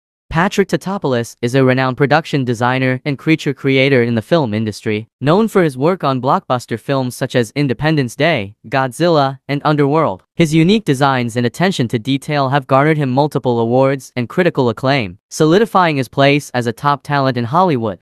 The Incredible Designs of Patrick Tatopoulos.
In this video, we explore the incredible designs of renowned artist and production designer, Patrick Tatopoulos. Known for his work on blockbuster films such as "Independence Day" and "Underworld," Tatopoulos continues to push the boundaries of creativity and innovation in the world of visual storytelling. Join us as we delve into his mesmerizing work and discover the magic behind his iconic designs. From otherworldly creatures to futuristic landscapes, Tatopoulos's imagination knows no limits. Don't miss out on this exclusive behind-the-scenes look at the mastermind behind some of Hollywood's most unforgettable visuals.